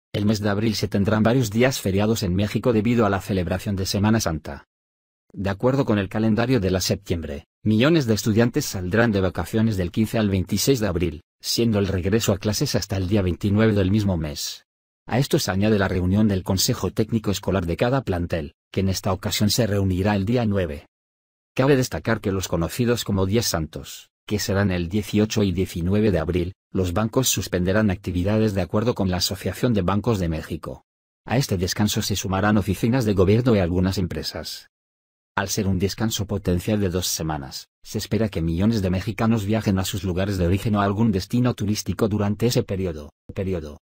Estos serán los días feriados en Abril 2019
El mes de abril se tendrán varios días feriados en México debido a la celebración de Semana Santa.

De acuerdo con el calendario de la SEP, millones de estudiantes saldrán de vacaciones del 15 al 26 de abril, siendo el regreso a clases hasta el día 29 del mismo mes. A esto se añade la reunión del Consejo Técnico Escolar de cada plantel, que en esta ocasión se reunirá el día 9.

Cabe destacar que los conocidos como Días Santos, que serán el 18 y 19 de abril, los bancos suspenderán actividades de acuerdo con la Asociación de Bancos de México. A este descanso se sumarán oficinas de gobierno y algunas empresas.

Al ser un descanso potencial de dos semanas, se espera que millones de mexicanos viajen a sus lugares de origen o a algún destino turístico durante ese periodo.